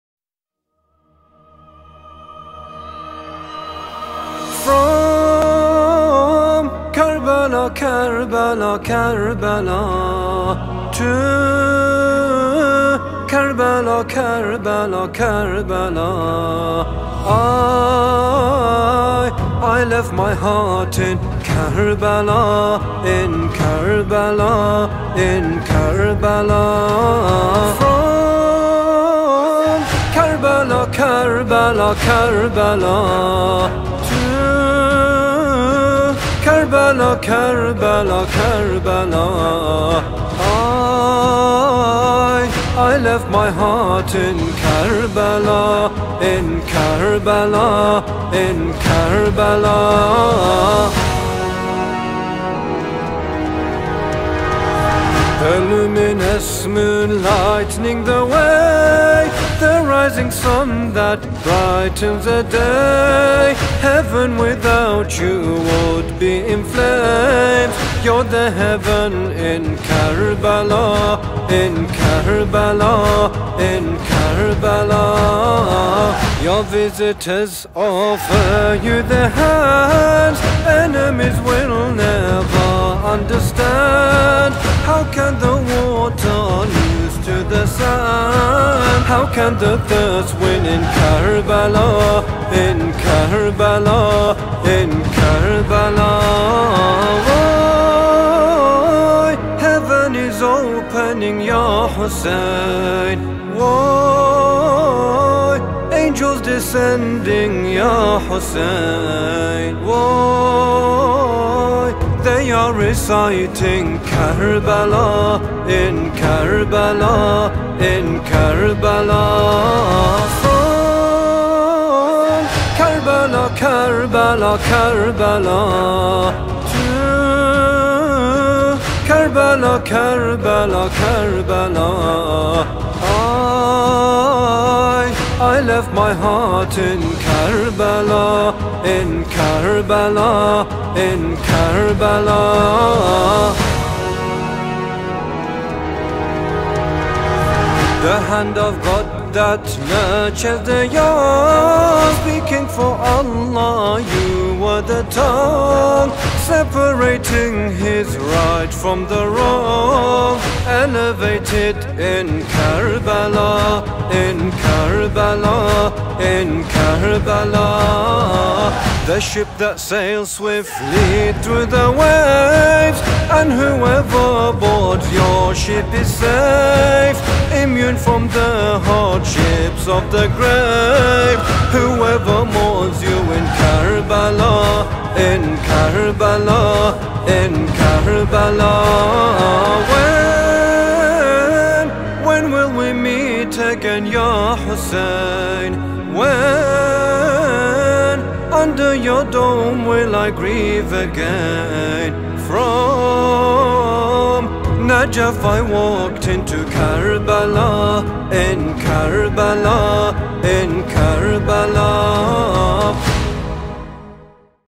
Recap - Muharram 2020 @ Saba Islamic Center
A recap of the Tabarruk Drive Thru's and the Live Outdoor Majlais.

Watch the full majlis on our Youtube channels (for each language).